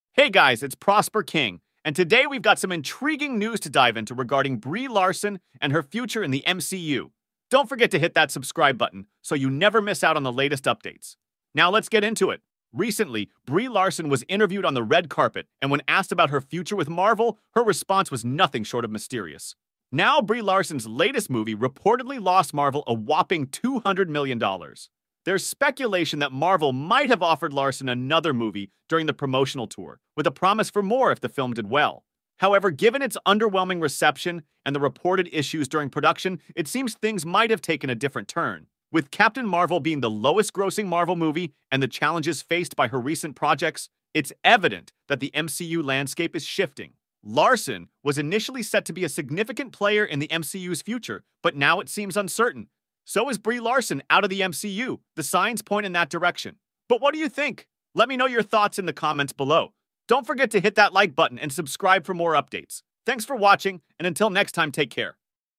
Brie Larson FIRED From Captain Marvel?! | Latest MCU Updates.
Hey, it's Prosper King, and today we're diving into some intriguing developments surrounding Brie Larson and her potential departure from the Marvel Cinematic Universe.

In a recent red carpet interview, Larson's response to questions about her future with Marvel was cryptic, leaving fans wondering what's next for the acclaimed actress. We'll break down the details and explore the financial rollercoaster of her latest movie, which reportedly lost Marvel a staggering $200 million.

Rumors are swirling that Marvel might have extended another movie offer during the promotional tour, but with underwhelming reception and production challenges, it seems the situation has taken an unexpected turn.

Join me as we dissect the shifting landscape of the MCU, with Captain Marvel's status as the lowest-grossing Marvel movie and the uncertainties surrounding Larson's role in future projects. Is Brie Larson on her way out of the MCU? The signs suggest so, but I want to hear YOUR thoughts!